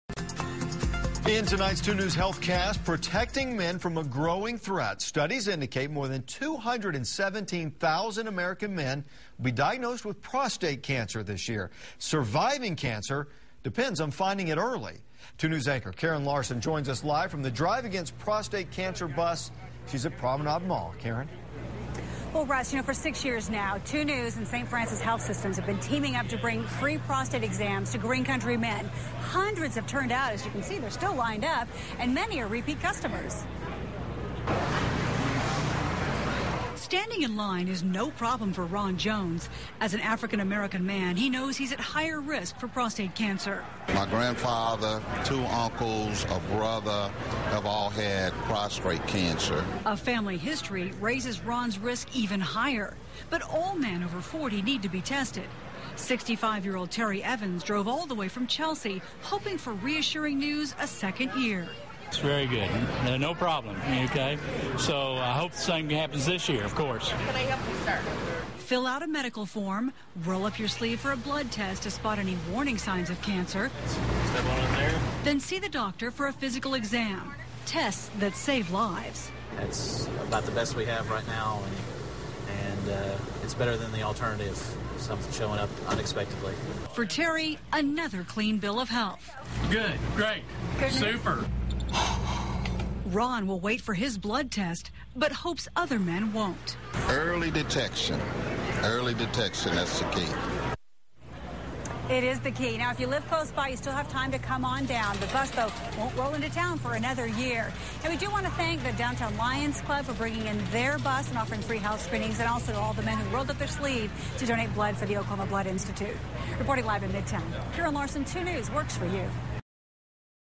Saving lives: free prostate exams
2News and Saint Francis Health Systems bring the "Drive Against Prostate Cancer Bus" back to Tulsa Friday.